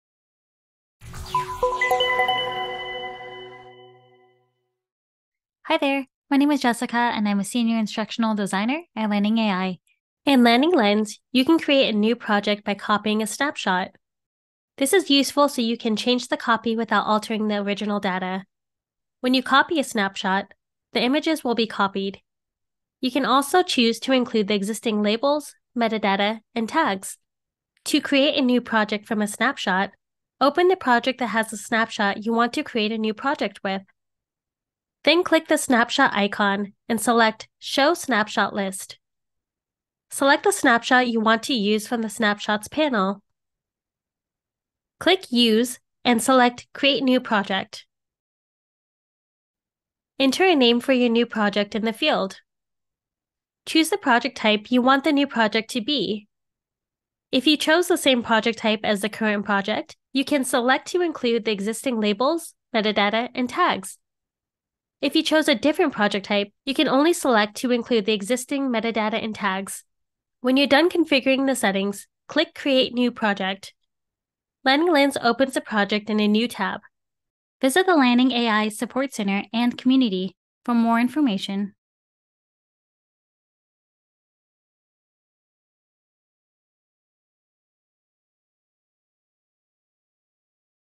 Create New Projects from Snapshots
In LandingLens, you can create a new project by copying a snapshot. This is useful so you can change the copy without altering the original data. Check out this tutorial video to learn more.